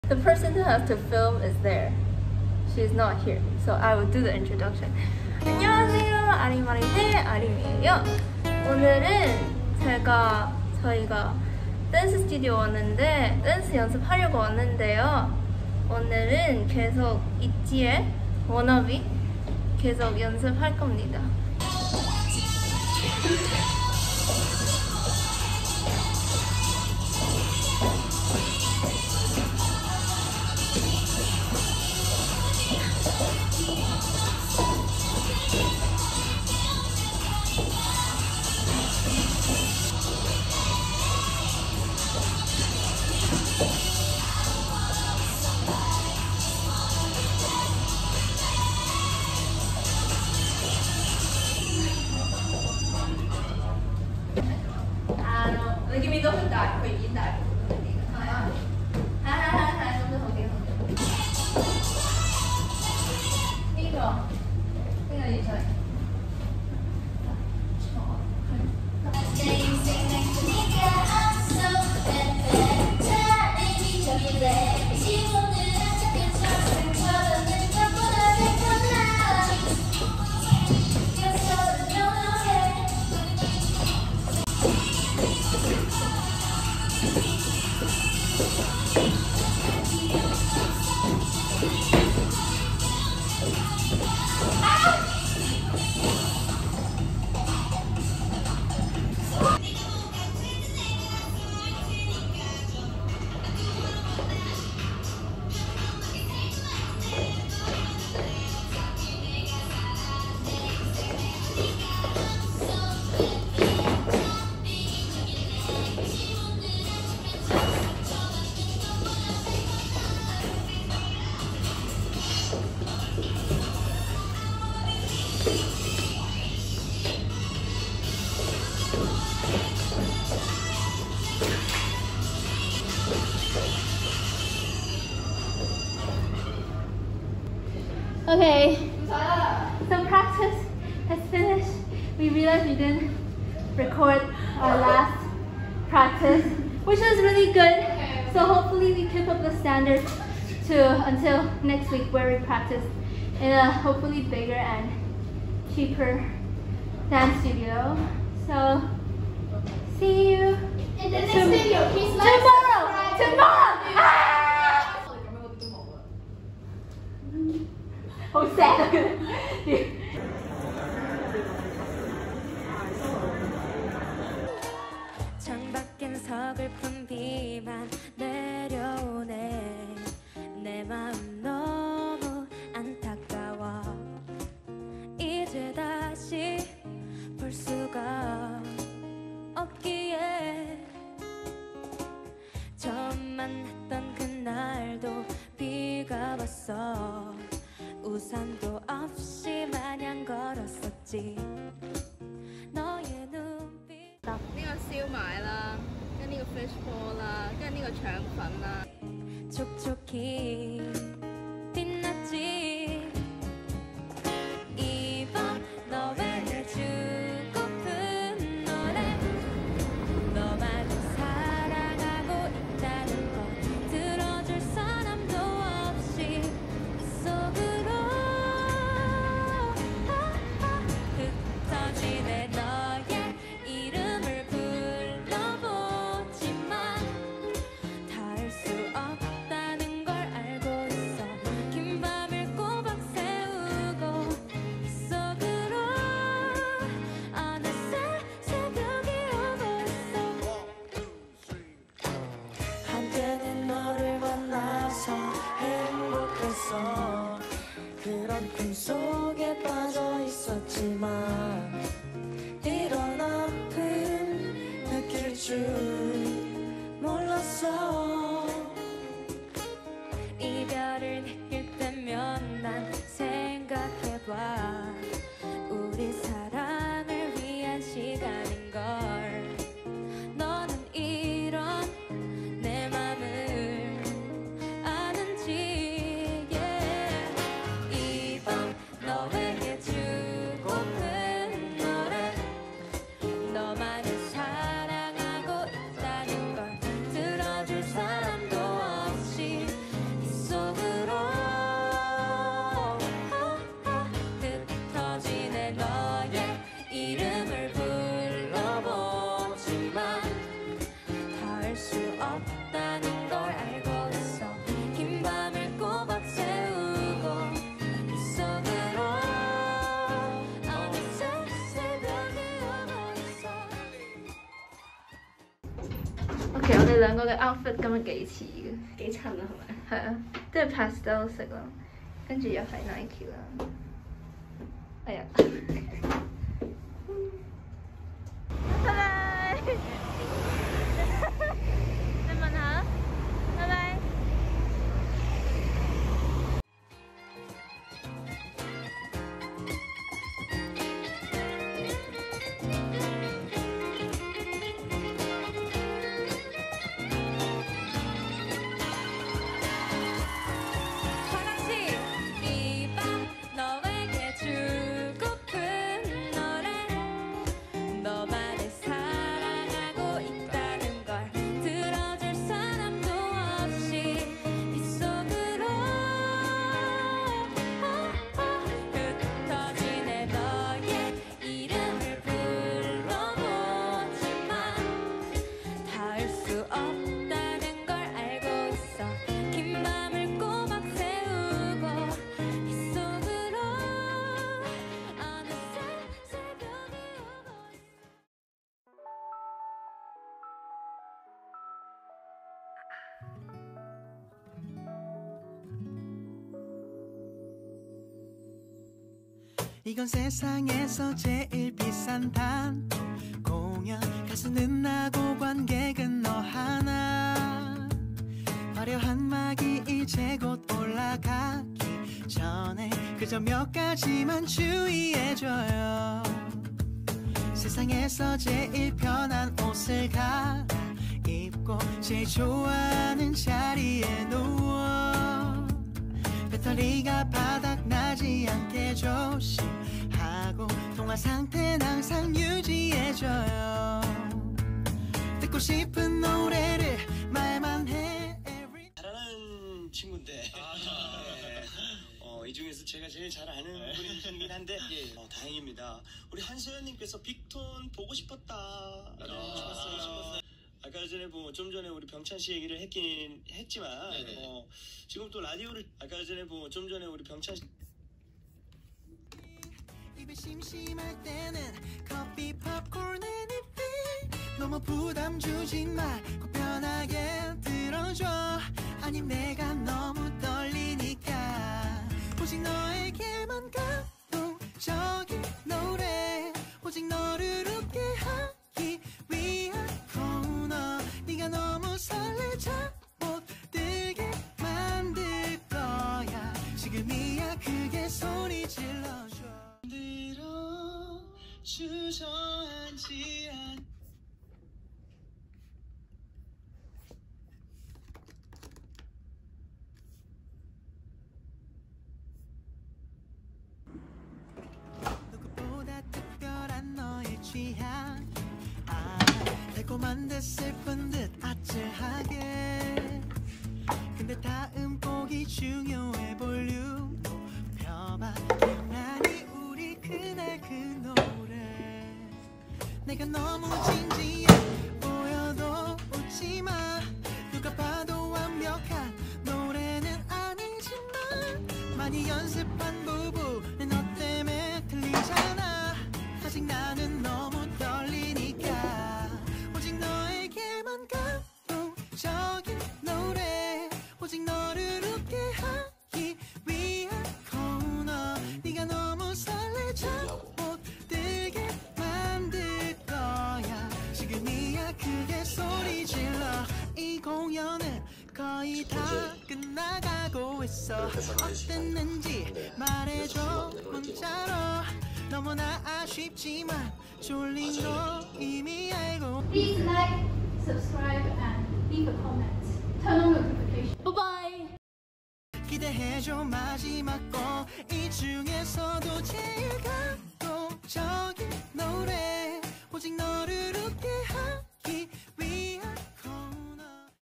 홍콩 01년생의 일주일 브이로그 [댄스연습/와구와구/방탄팬아트/한국어공부]
친구들과 함께 행복하게 보냈던 일주일의 시간! 제가 홍콩에서 어떻게 지내고 있는지 보여줍니다!